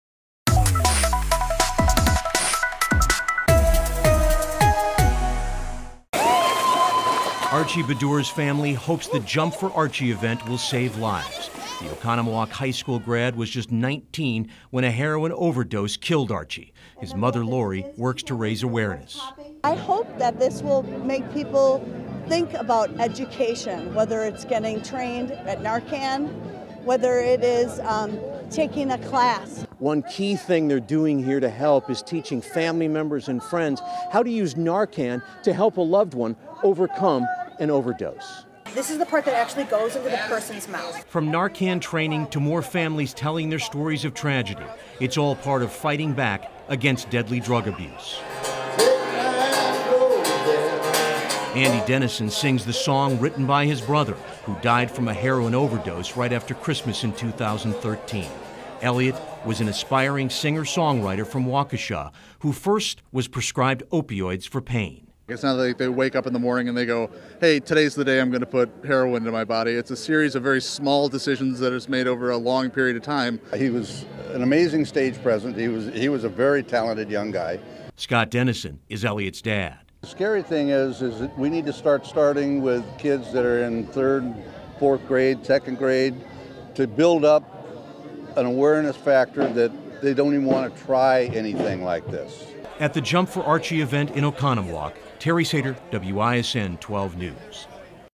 Opioid Epidemic Takes Center Stage At Oconomowoc Festival
​The "Jump for Archie" celebrates the life of Oconomowoc High School graduate Archie Badura, who was just 19 years old when he died of an opioid overdose, and aims to raise awareness of the epidemic.